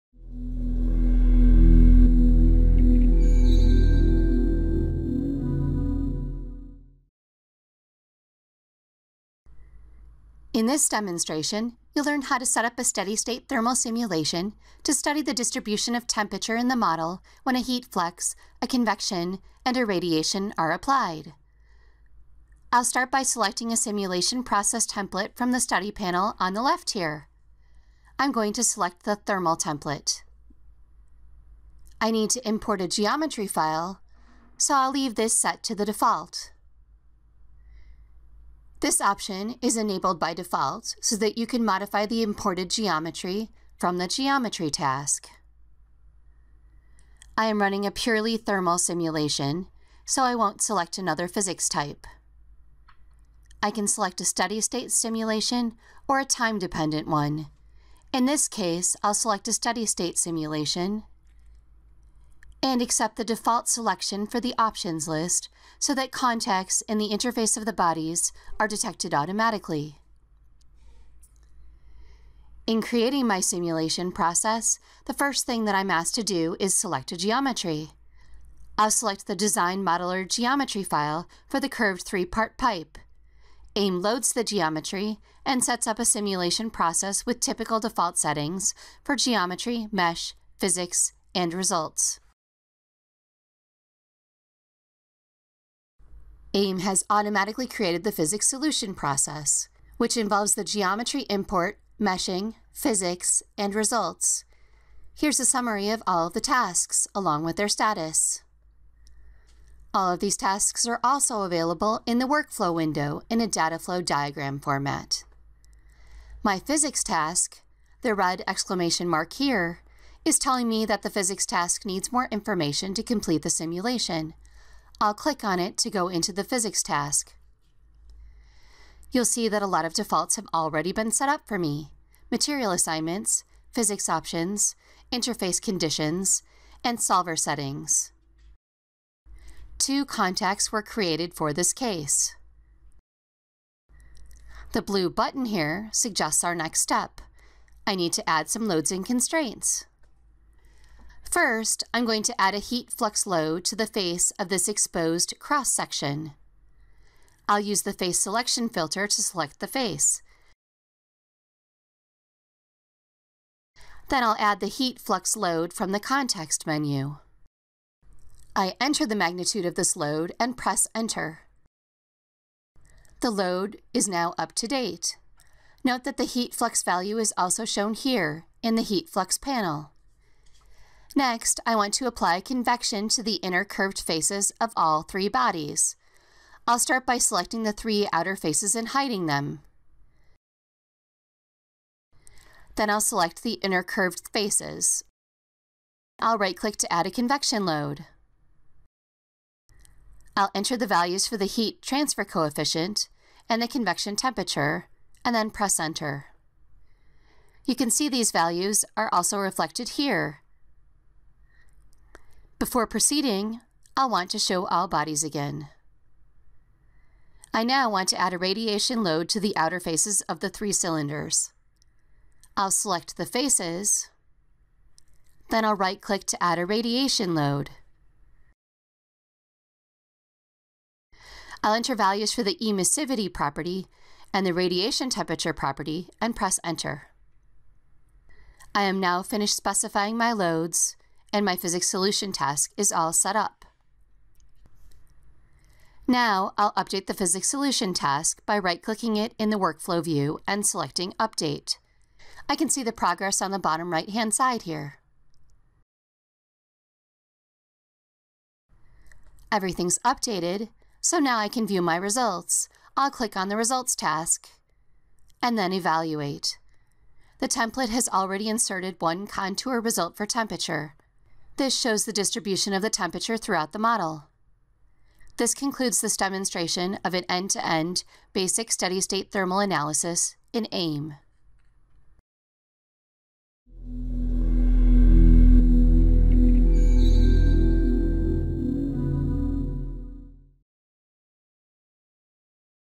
ANSYS AIM: Steady-State Thermal Physics Overview
This video demonstrates the workflow for a steady-state thermal simulation in ANSYS AIM 18.0. ANSYS AIM provides easy simulation software for design engineering in an integrated multiphysics simulation environment.